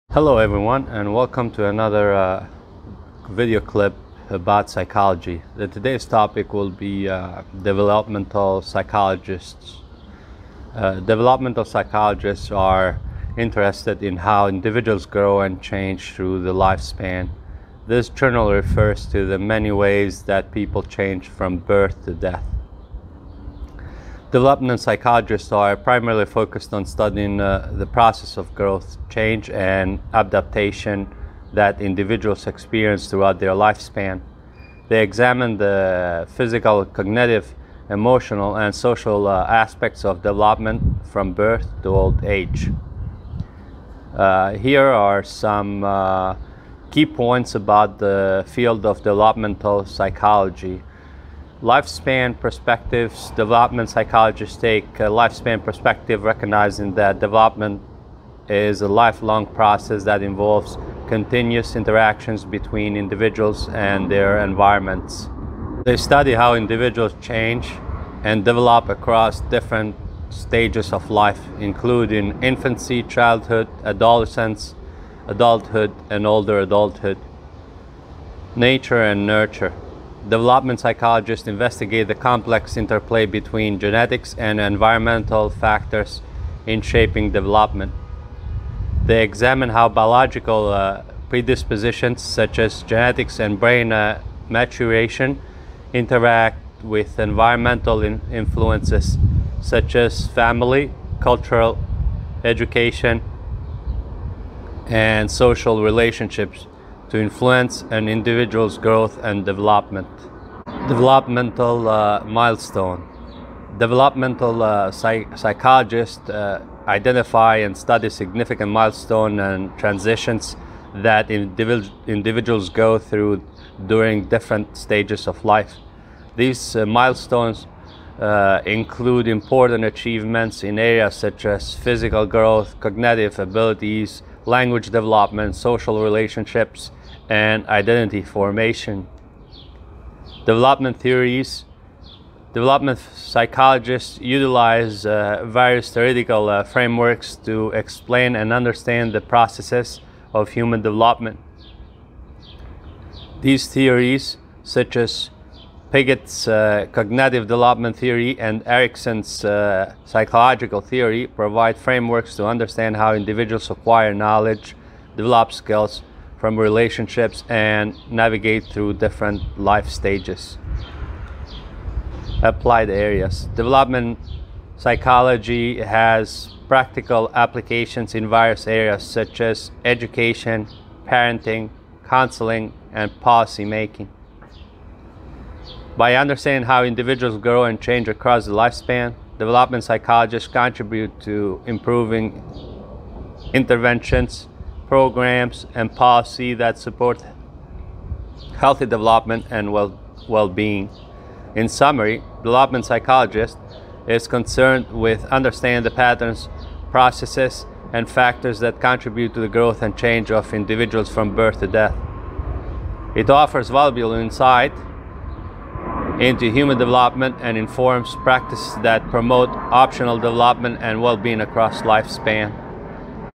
Developmental psychologists: from womb to tomb!.
Developmental psychologists: are interested in how individuals grow and change throughout the lifespan. this generally refers to the many ways that people change from birth to death.

Developmental psychologists are primarily focused on studying the processes of growth, change, and adaptation that individuals experience throughout their lifespan.

They examine the physical, cognitive, emotional, and social aspects of development from birth to old age.

Here are some key points about the field of developmental psychology:

Lifespan Perspective, Developmental psychologists take a lifespan perspective, recognizing that development is a lifelong process that involves continuous interactions between individuals and their environments.

They study how individuals change and develop across different stages of life, including infancy, childhood, adolescence, adulthood, and older adulthood.

Nature and Nurture, Developmental psychologists investigate the complex interplay between genetic and environmental factors in shaping development.

They examine how biological predispositions, such as genetics and brain maturation, interact with environmental influences, such as family, culture, education, and social relationships, to influence an individual's growth and development.

Developmental Milestones, Developmental psychologists identify and study significant milestones and transitions that individuals go through during different stages of life.

These milestones include important achievements in areas such as physical growth, cognitive abilities, language development, social relationships, and identity formation.

Developmental Theories, Developmental psychologists utilize various theoretical frameworks to explain and understand the processes of human development.

These theories, such as Piaget's cognitive development theory and Erikson's psychosocial theory, provide frameworks for understanding how individuals acquire knowledge, develop skills, form relationships, and navigate through different life stages.

Applied Areas, Developmental psychology has practical applications in various areas, such as education, parenting, counseling, and policy-making.

By understanding how individuals grow and change across the lifespan, developmental psychologists contribute to improving interventions, programs, and policies that support healthy development and well-being.

In summary, developmental psychology is concerned with understanding the patterns, processes, and factors that contribute to the growth and change of individuals from birth to death.

It offers valuable insights into human development and informs practices that promote optimal development and well-being across the lifespan.